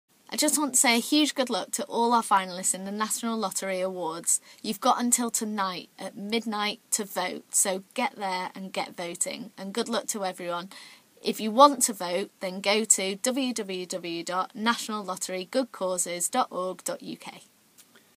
Beth Tweddle wishing Good luck to all finalists of the National Lottery Awards
NLAwards Time is running out to get that all important vote in, here’s a very special National Lottery Awards message from Britain’s greatest ever female gymnast Beth Tweddle vote